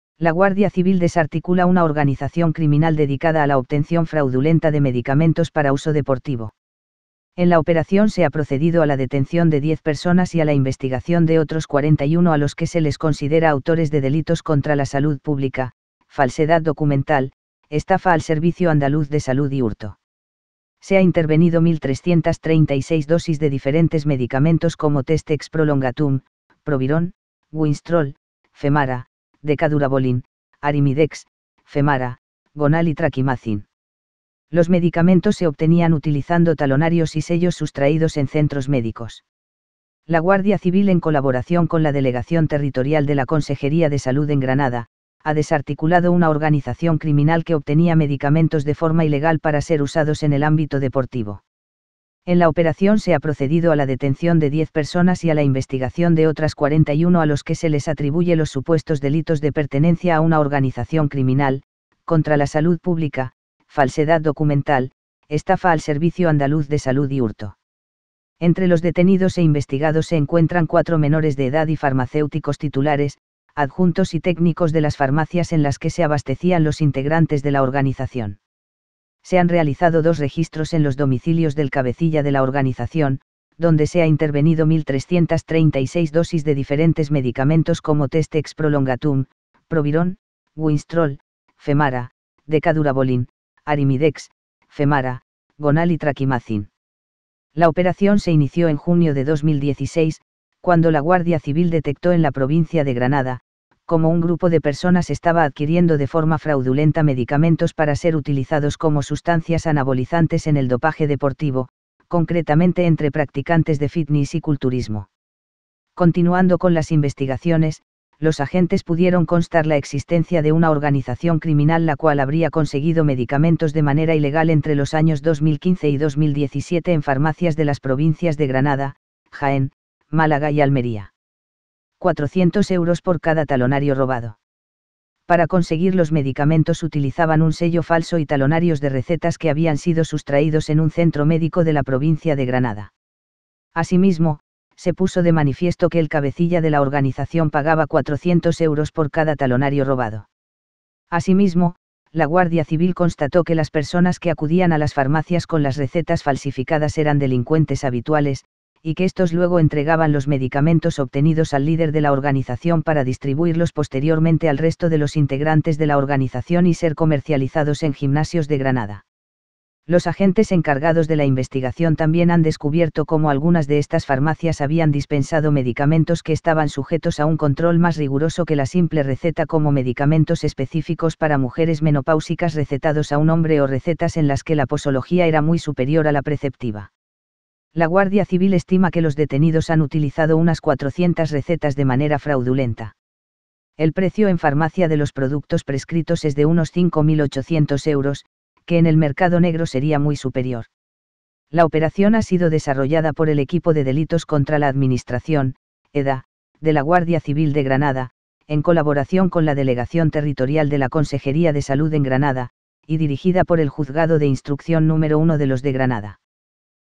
Robaban talonarios para medicamentos deportivos en Granada
La Guardia Civil desarticula una organización criminal dedicada a la obtención fraudulenta de medicamentos para uso deportivo

En la operación se ha procedido a la detención de 10 personas y a la investigación de otros 41 a los que se les considera autores de delitos contra la salud pública, falsedad documental, estafa al Servicio Andaluz de Salud y hurto

Se ha intervenido 1.336 dosis de diferentes medicamentos como Testex prolongatum, Provirón, Winstrol, Femara, Decadurabolin, Arimidex, femara, Gonal y Trakimazin

Los medicamentos se obtenían utilizando talonarios y sellos sustraídos en centros médicos

La Guardia Civil en colaboración con la Delegación Territorial de la Consejería de Salud en Granada, ha desarticulado una organización criminal que obtenía medicamentos  de forma ilegal para ser usados en el ámbito deportivo. En la operación se ha procedido a la detención de 10 personas y a la investigación de otras 41 a los que se les atribuye los supuestos delitos de pertenencia a una organización criminal, contra la salud pública, falsedad documental, estafa al Servicio Andaluz de Salud y hurto.

Entre los detenidos e investigados se encuentran cuatro menores de edad y farmacéuticos titulares, adjuntos y técnicos de las farmacias en las que se abastecían los integrantes de la organización.

Se han realizado dos registros en los domicilios del cabecilla de la organización, donde se ha intervenido 1.336 dosis de diferentes medicamentos como Testex prolongatum, Provirón, Winstrol, Femara, Decadurabolin, Arimidex, femara, Gonal y Trakimazin.

La operación se inició en junio de 2016, cuando la Guardia Civil detectó  en la provincia de Granada, como un grupo de personas estaba adquiriendo de forma fraudulenta medicamentos para ser utilizados como sustancias anabolizantes en el dopaje deportivo, concretamente entre practicantes de fitness y culturismo.

Continuando con las investigaciones, los agentes pudieron constar la existencia de una organización criminal la cual habría conseguido medicamentos de manera ilegal entre los años 2015 y 2017 en farmacias de las provincias de Granada, Jaén, Málaga y Almería.

400 euros por cada talonario robado
Para conseguir los medicamentos utilizaban un sello falso y talonarios de recetas que habían sido sustraídos en un centro médico de la provincia de Granada. Asimismo, se puso de manifiesto que el cabecilla de la organización pagaba 400 euros por cada talonario robado.

Asimismo, la Guardia Civil constató que las personas que acudían a las farmacias con las recetas falsificadas eran delincuentes habituales, y que estos luego entregaban los medicamentos obtenidos al líder de la organización para distribuirlos posteriormente al resto de los integrantes de la organización y ser comercializados en gimnasios de Granada.

Los agentes encargados de la investigación también han descubierto como algunas de estas farmacias habían dispensado medicamentos que estaban sujetos a un control más riguroso que la simple receta como medicamentos específicos para mujeres menopáusicas recetados a un hombre o recetas en las que la posología era muy superior a la preceptiva.

La Guardia Civil estima que los detenidos han utilizado unas 400 recetas de manera fraudulenta. El precio en farmacia de los productos prescritos es de unos 5.800 euros, que en el mercado negro sería muy superior.

La operación ha sido desarrollada por el Equipo de Delitos Contra la Administración (EDA) de la Guardia Civil de Granada, en colaboración con la Delegación Territorial de la Consejería de Salud en Granada, y dirigida por el Juzgado de Instrucción nº 1 de los de Granada.